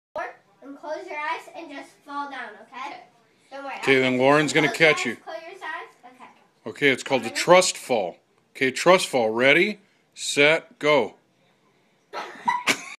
Trust fall fail - Nederlandse ondertiteling
Thanks to CrunchGamer1's channel for the video! I couldn't find Dutch subtitles, so I made them myself for our Dutch audience.